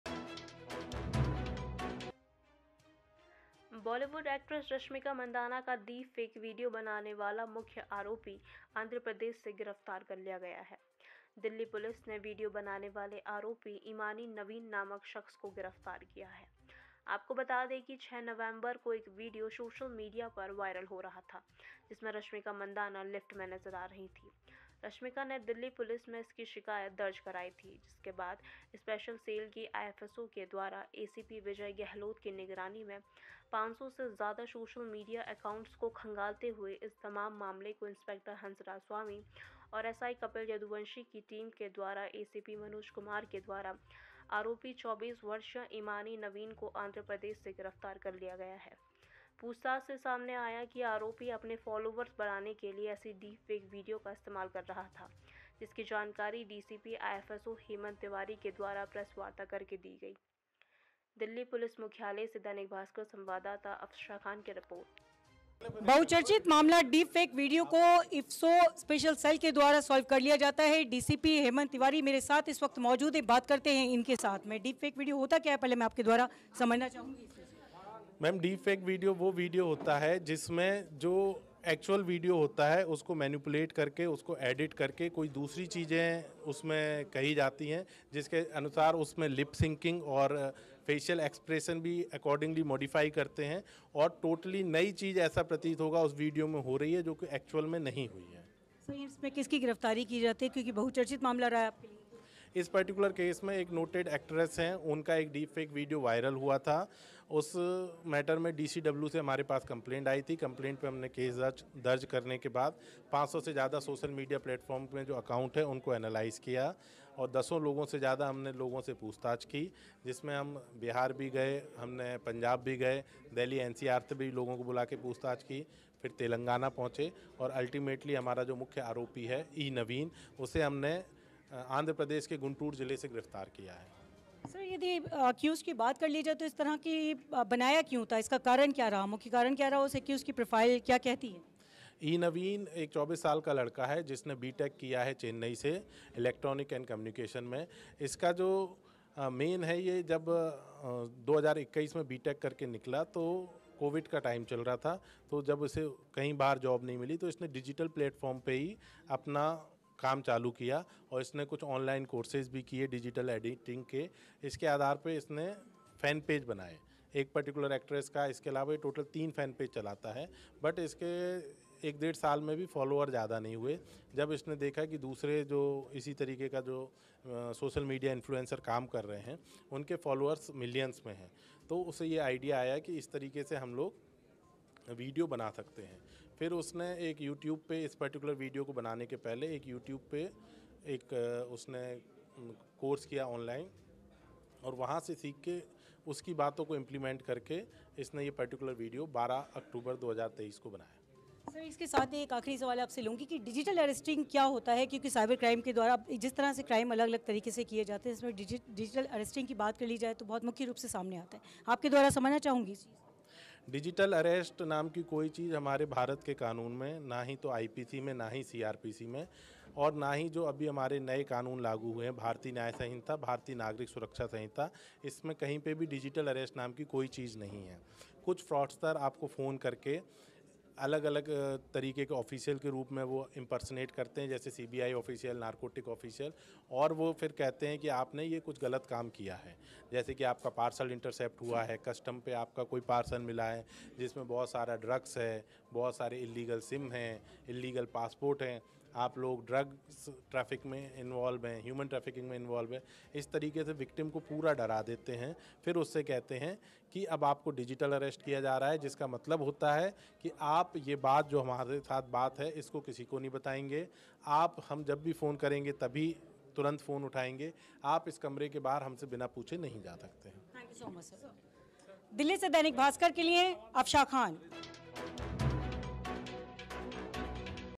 दिल्ली : बहुचर्चित बॉलीवुड एक्ट्रेस के डीप फेक मामले को स्पेशल सेल की इफसो द्वारा सुलझाया गया
बॉलीवुड एक्ट्रेस रश्मिका मंदांना का डीप फेक वीडियो बनाने वाला मुख्य आरोपी आंध्र प्रदेश से गिरफ्तार कर लिया गया है दिल्ली पुलिस ने वीडियो बनाने वाले आरोपी ईमानी नवीन नामक शख्स को गिरफ्तार किया है आपको बता दें कि  6 नवंबर को एक वीडियो सोशल मीडिया पर वायरल हुआ था जिसमें रश्मिका मंदाना लिफ्ट में नजर आ रही हैं। रश्मिका ने दिल्ली पुलिस में इसकी शिकायत दर्ज कराई थी। जिसके बाद स्पेशल सेल की इफसो के द्वारा एसीपी    विजय गेहलावत की निगरानी मे 500 से ज़्यादा सोशल मीडिया एकाउंट्स को खंगालते हुए इस तमाम मामले को इंस्पेक्टर हंसराज स्वामी और एस आई कपिल यदुवंशी की टीम के द्वारा एसीपी मनोज कुमार के द्वारा आरोपी 24 वर्षीय ईमानी नवीन को आंद्र प्रदेश से गिरफ्तार कर लिया गया है पूछताछ से सामने आया की आरोपी अपने फॉलोवर बढ़ाने के लिए ऐसी डीप् फेक   वीडियो का इस्तेमाल कर रहा था जिसकी जानकारी डीसीपी इफसो हेमंत तिवारी के द्वारा प्रेस वार्ता कर दी गयी । देखिये दैनिक भास्कर संवाददाता अफशा खान की रिपोर्ट।